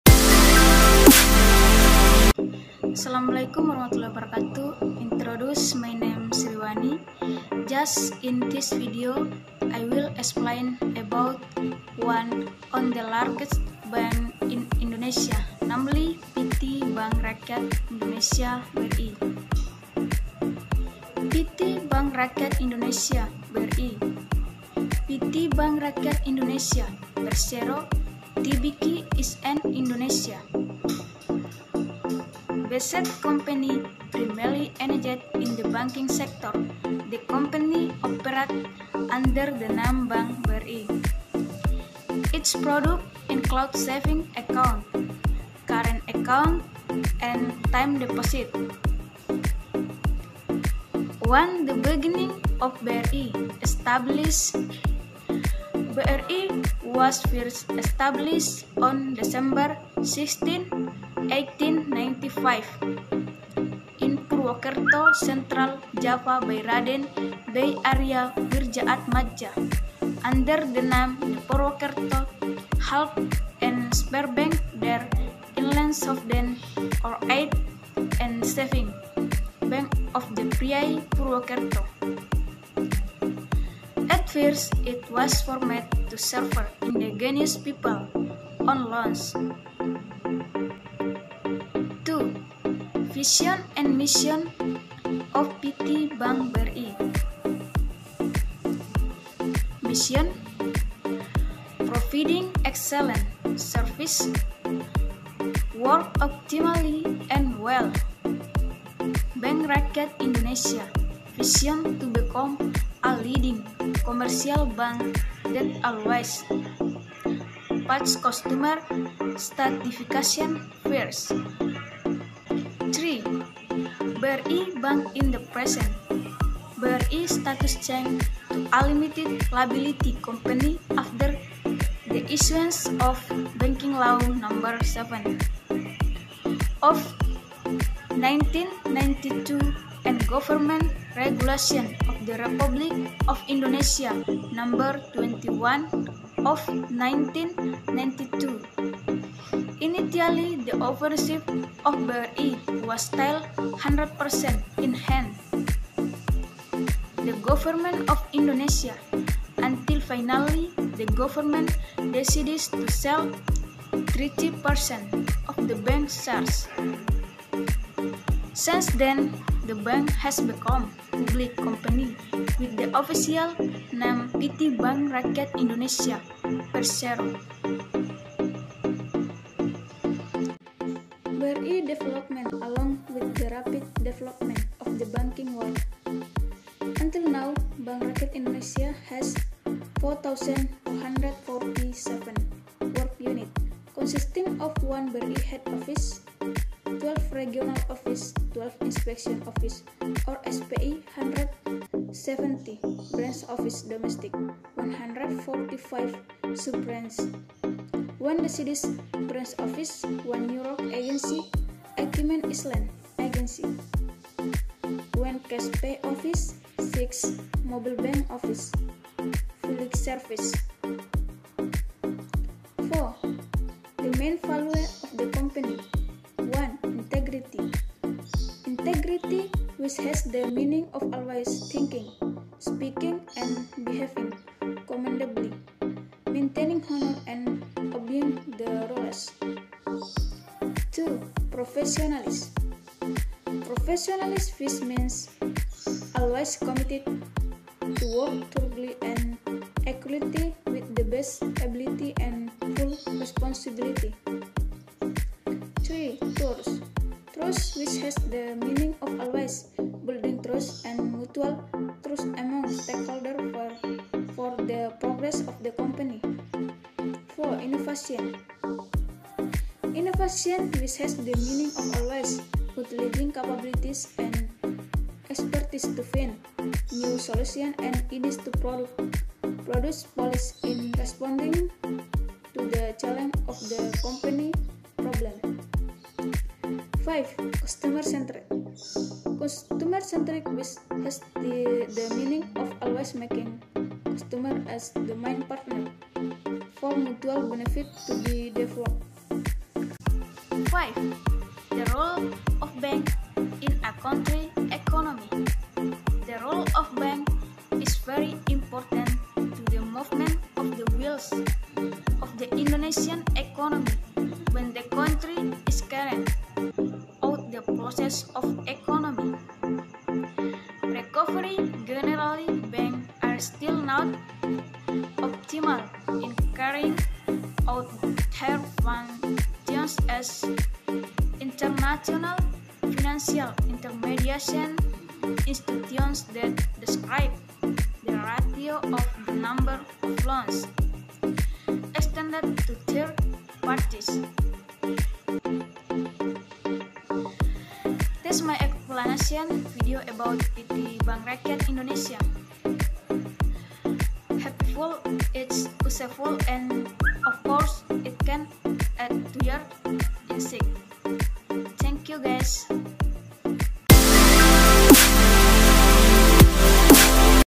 Presentasi PT Bank Rakyat Indonesia (Persero) TBK.
*𝗕𝗮𝗻𝗸 𝗥𝗮𝗸𝘆𝗮𝘁 𝗜𝗻𝗱𝗼𝗻𝗲𝘀𝗶𝗮 (𝗕𝗥𝗜)

  Bank Rakyat Indonesia (BRI)is one of the largest state-owned banks in Indonesia. Bank Rakyat Indonesia (BRI) was established in Purwokerto, Central Java by Raden Bei Aria Wirjaatmadja on December 16, 1895.